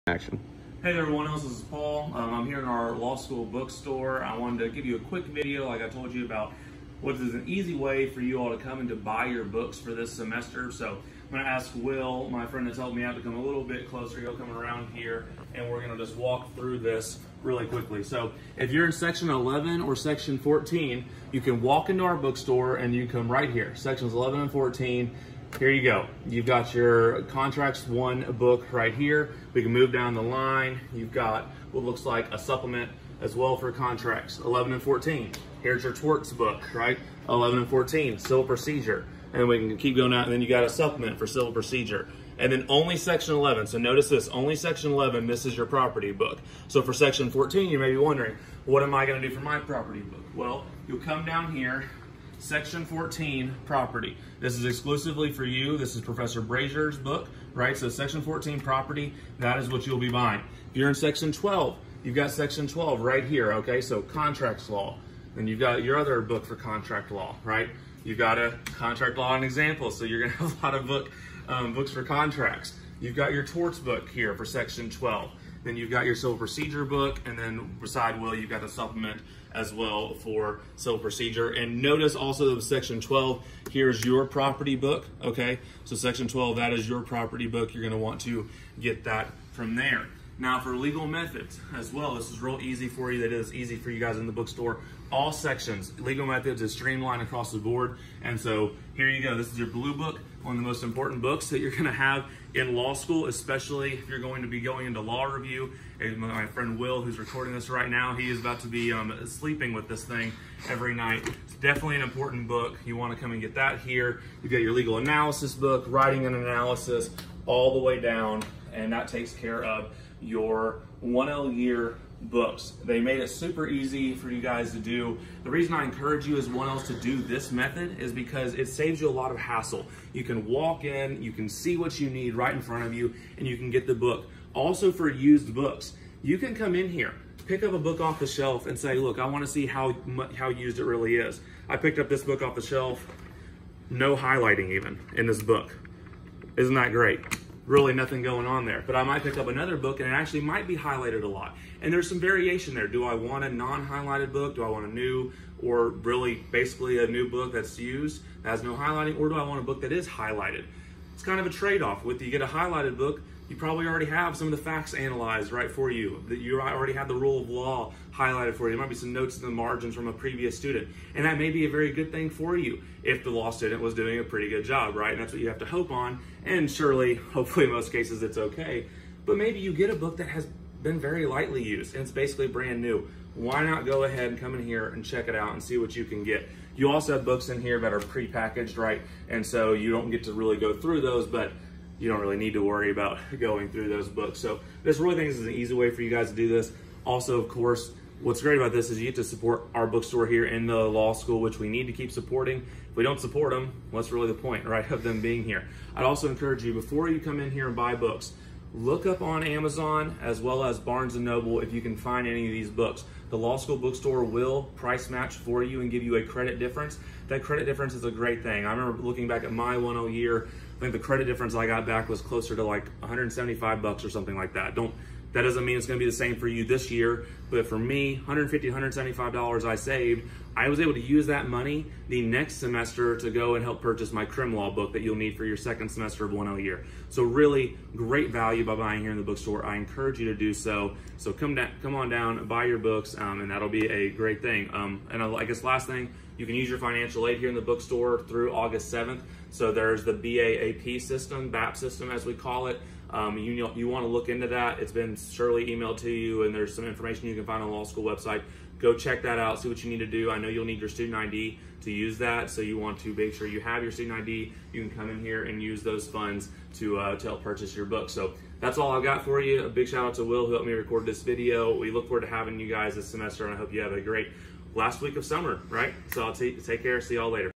Memphis Law Bookstore Walkthrough
In this video Paul will explain how easy it is for Memphis Law 1Ls to find and purchase law school books through the Memphis Law bookstore. This video does not cover buying books through cecilslist on Facebook.